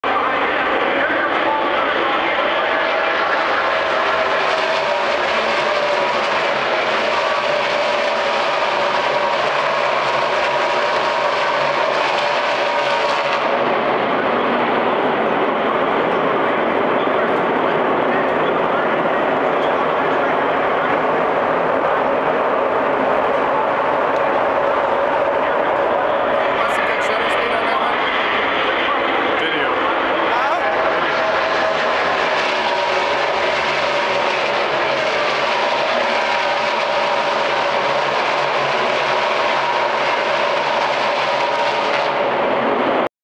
NASCAR - Subway Fresh Fit 500 - PIR Phoenix Spring 2009
A view of the start finish line from the infield bridge above the entrace to the garage area